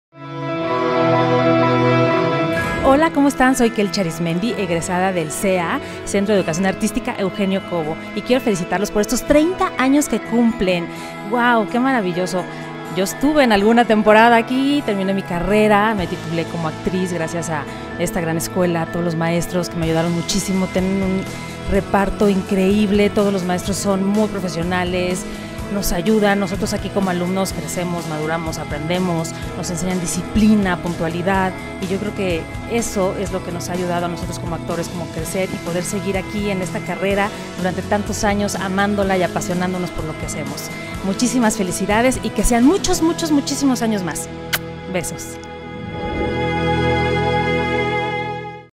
Kelchie Arizmendi
Muchas gracias a nuestra egresada Kelchie Arizmendi por su felicitación de 30AñosCEA